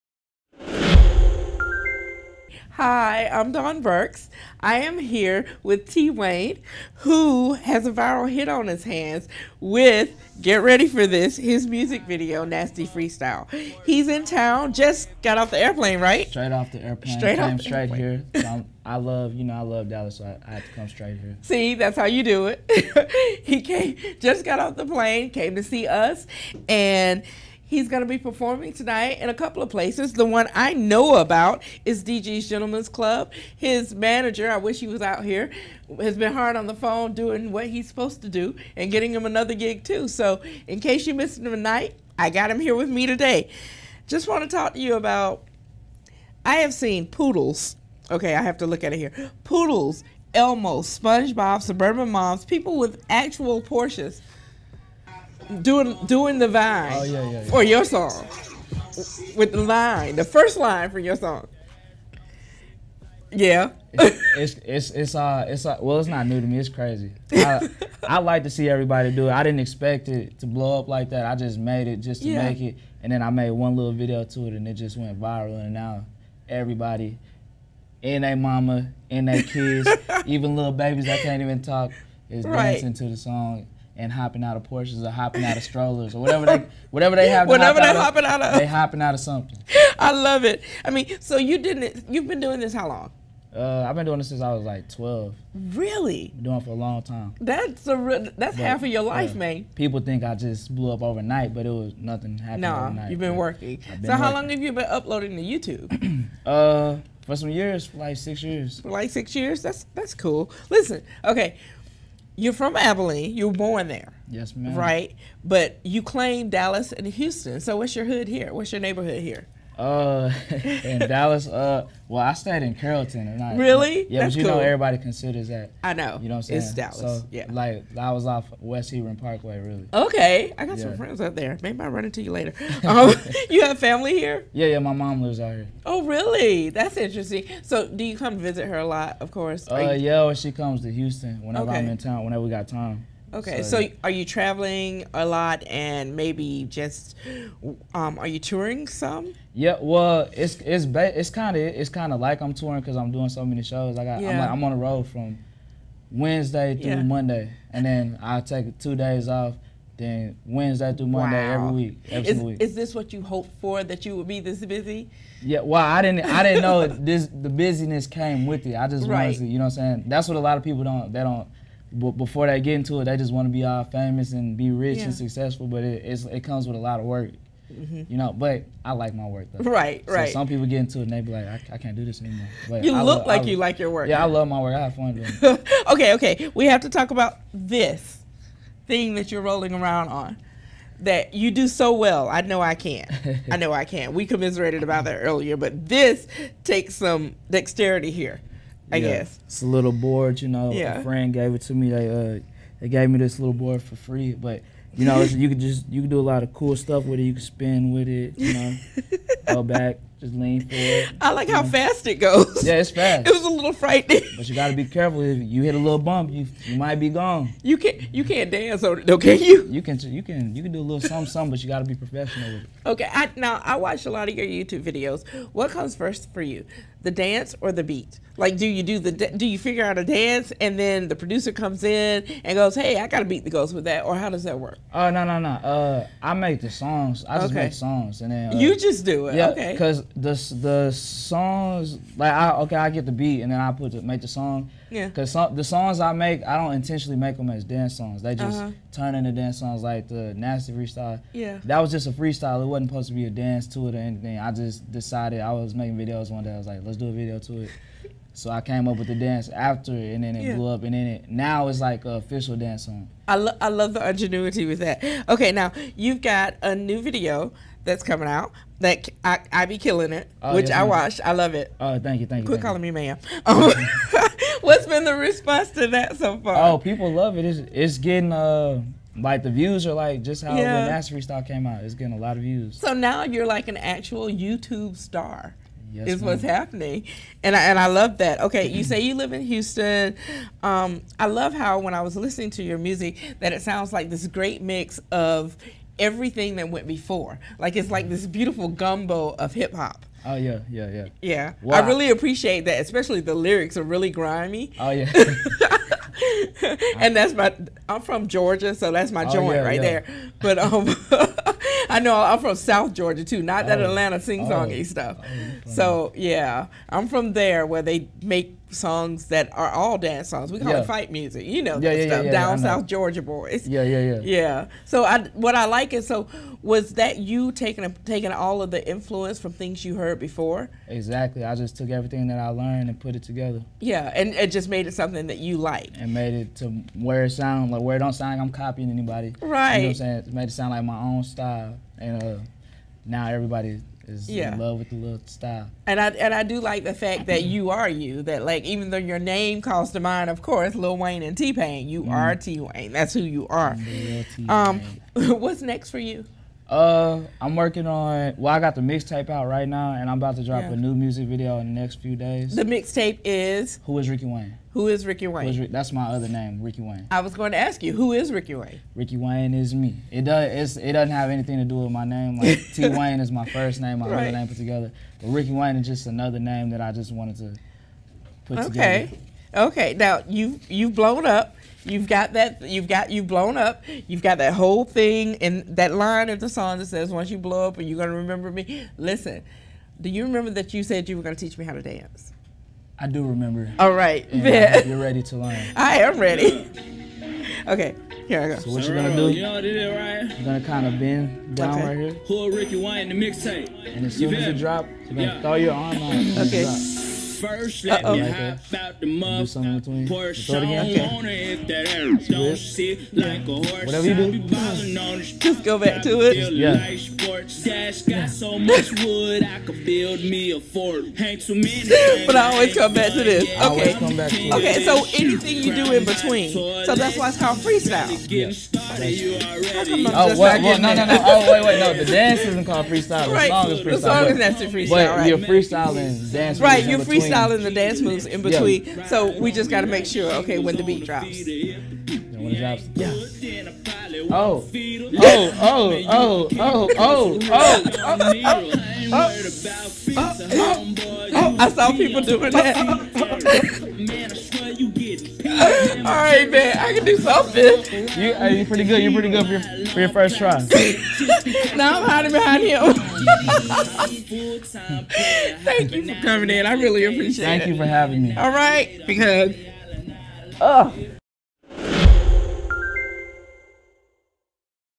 Freestyling with North Texas rapper T-Wayne
Dawn Burkes chats (and dances!) with North Texas rapper T-Wayne. Find out how he became an instant YouTube sensation.